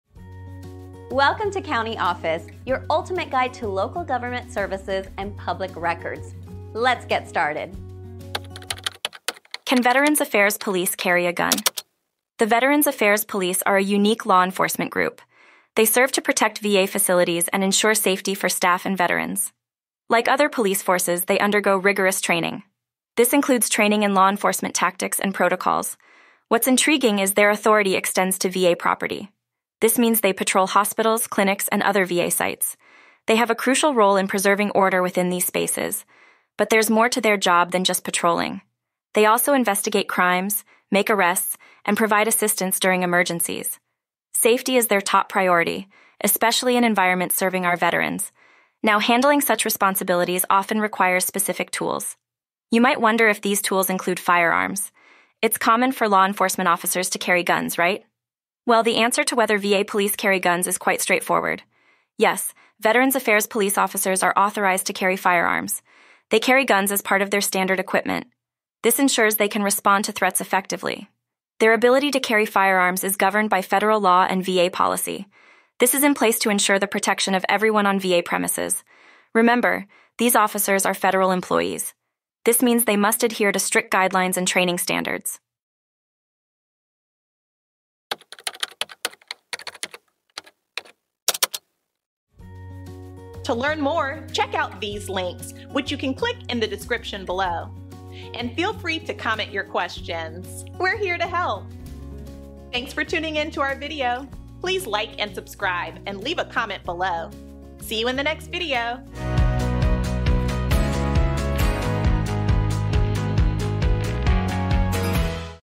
Can Veterans Affairs Police Carry A Gun? - CountyOffice.org
Can Veterans Affairs Police Carry A Gun? Explore the unique role of the Veterans Affairs Police in our latest video. These dedicated officers are tasked with the crucial job of protecting VA facilities and ensuring the safety of staff and veterans alike. Their responsibilities are vast, ranging from patrolling VA property to investigating crimes and responding to emergencies.

 The safety of those who have served our country is of utmost importance, and the VA Police are equipped with the necessary tools to maintain a secure environment. Our video delves into the specifics of their equipment and the federal laws and VA policies that govern their responsibilities.

 As federal employees, VA Police officers must adhere to stringent guidelines and training standards to perform their duties effectively. Tune in to discover more about how they fulfill their role and the measures in place to ensure the protection of everyone on VA premises.